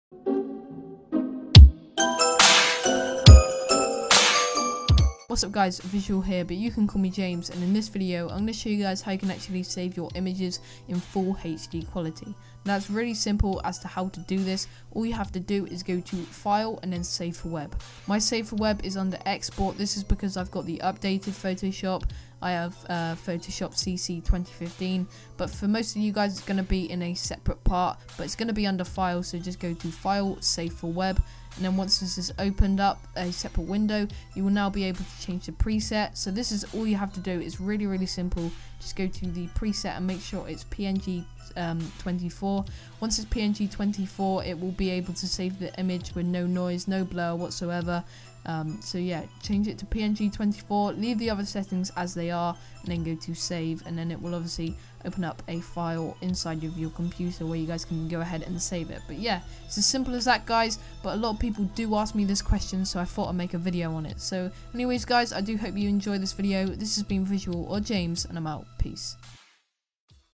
12 Days of Christmas #11: How to Save Designs In High Quality
Welcome to Day 11 of '12 Days of Christmas', where I show you 'How to Save Images In the Highest Quality Possible''...